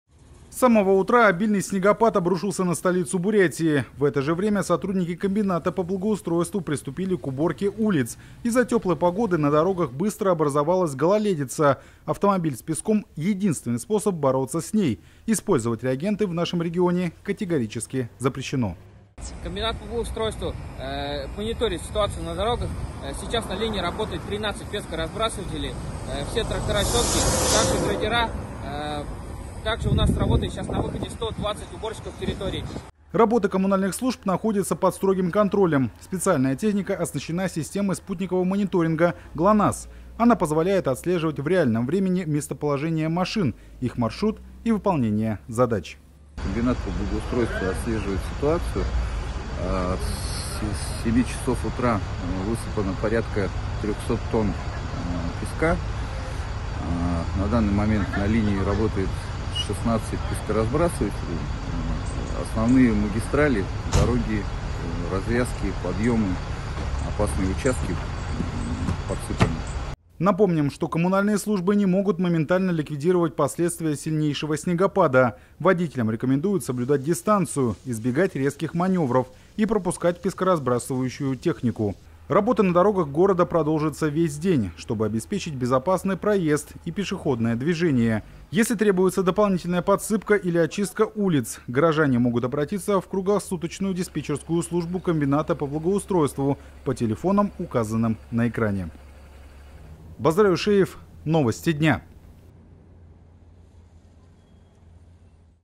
Снегопад в Улан-Удэ: коммунальные службы работают в усиленном режиме
В Улан-Удэ теплый атмосферный фронт принес снегопад и потепление. Это привело к образованию гололедицы на дорогах столицы. Специалисты Комбината по благоустройству с самого утра активно борются с последствиями непогоды.